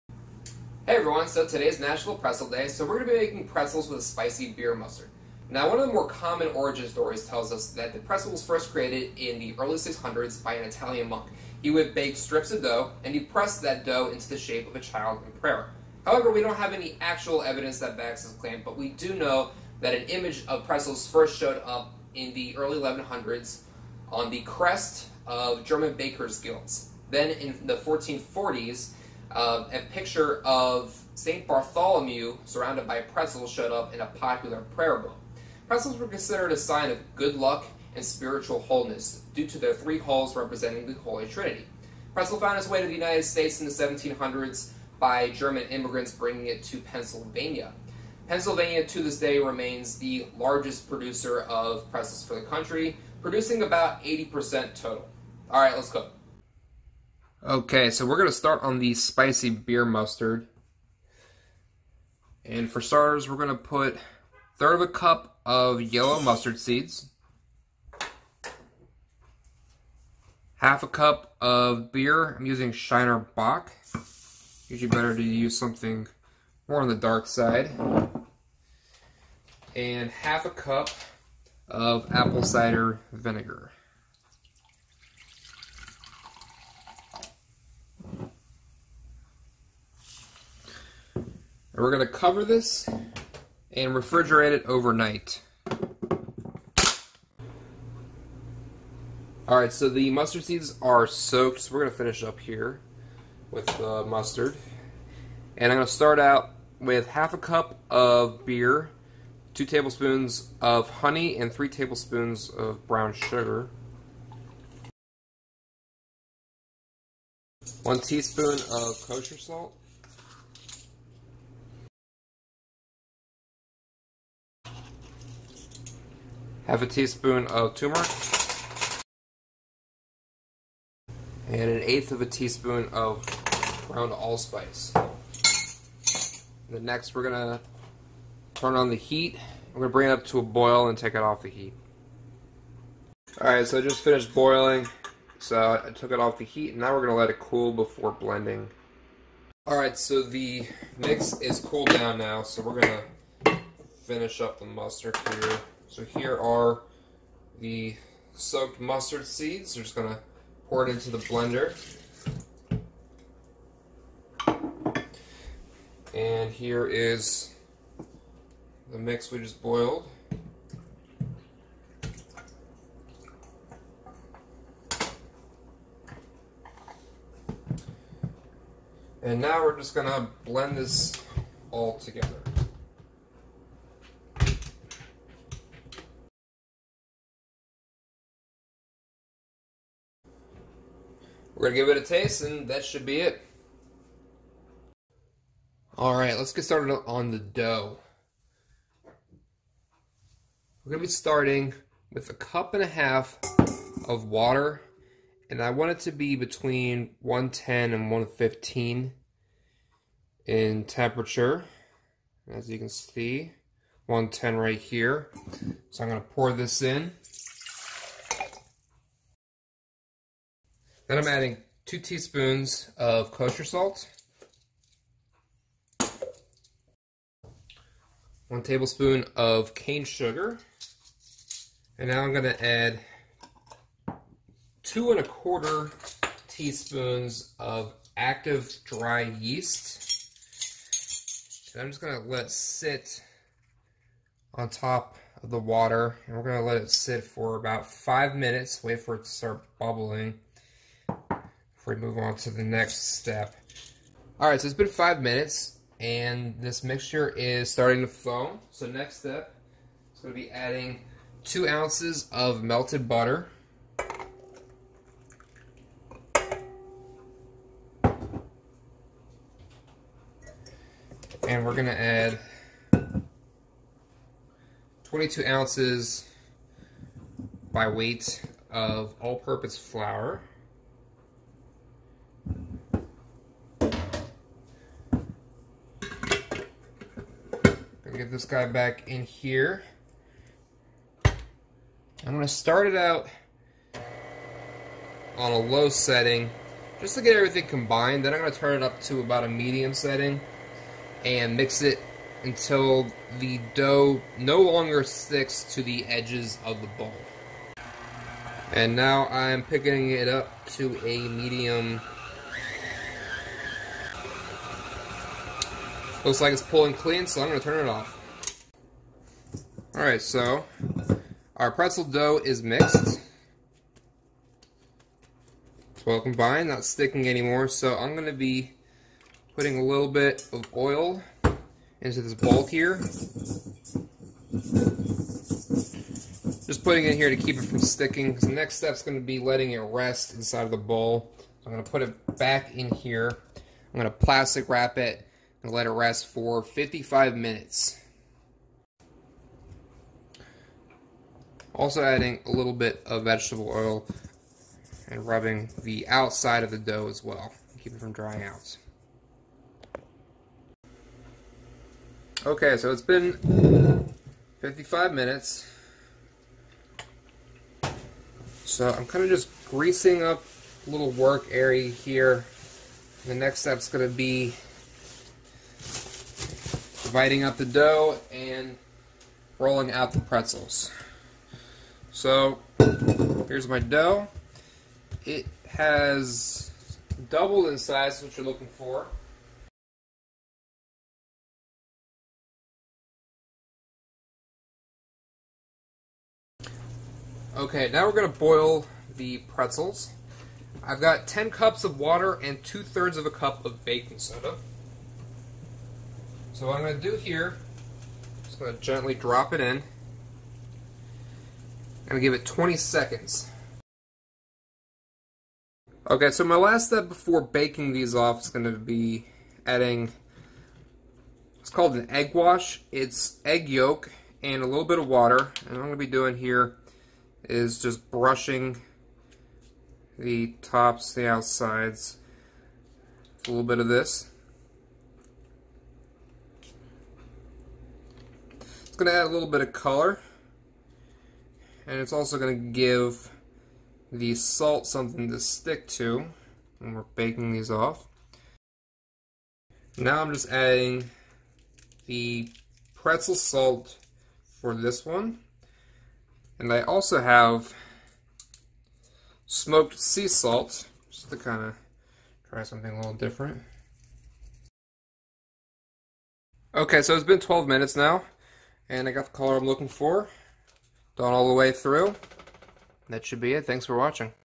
Soft Pretzels with Spicy Beer Mustard
Hey everyone, today is National Pretzel Day, so I'll be making pretzels, with a spicy beer mustard. As always, I'll talk a little bit about its origins, and how it made its way to the United States. I share Stanley Hudson's love for Pretzel Day, and I hope you do too!

Spicy Beer Mustard -
1/3 cup yellow mustard seeds
1/2 cup apple cider vinegar
1 cup dark beer, divided (i used Shiner Bock)
3 tbsp brown sugar
2 tbsp honey
1 tsp kosher salt
1/2 tsp turmeric
1/8 tsp ground cloves

Pretzel Dough -
1.5 cups water (110-115 F)
2 1/4 tsp active dry yeast
2 tsp kosher salt
1 tbsp cane sugar
2 oz. melted unsalted butter, cooled
22 oz. all purpose flour (by weight)

Boiling water ratio
10 cups water
2/3 cup baking soda

Garnish-
Coarse pretzel salt
Smoked sea salt